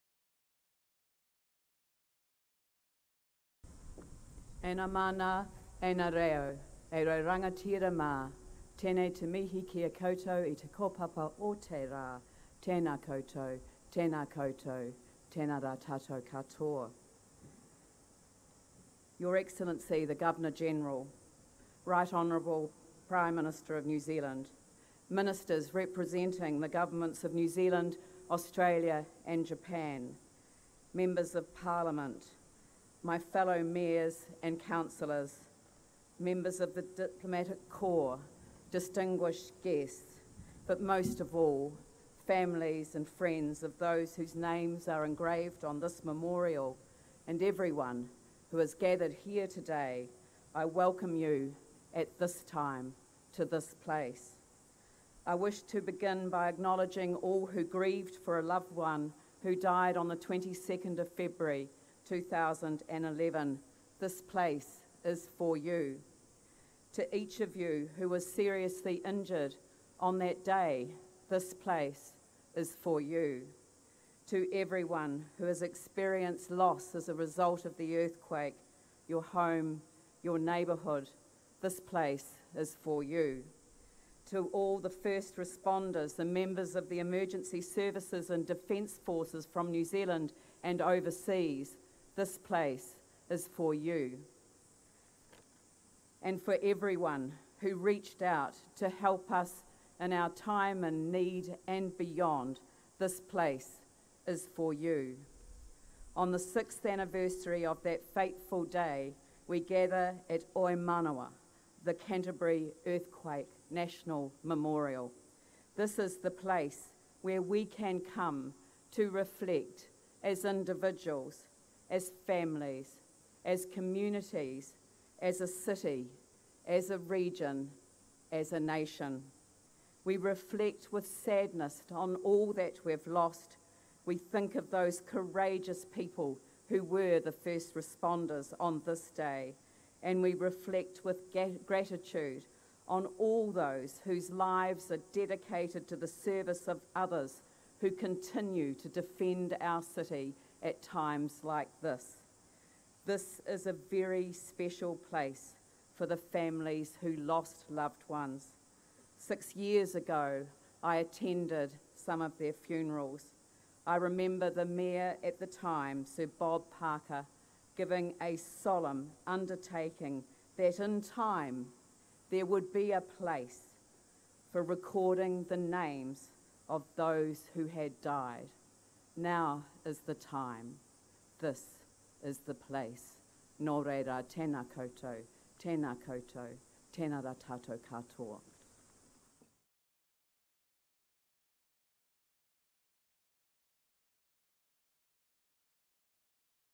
National Earthquake Memorial 2017 Christchurch - Lianne Dalziel address
Tandem Studios in association with Multicam Television